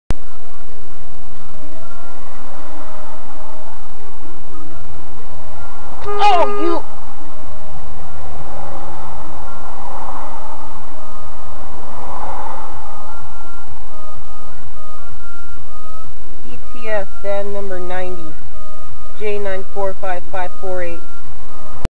ETS DATS Van Making Their Own Lane
ETS DATS van running the yellow light from a right hand turn only lane they blocking the intersection.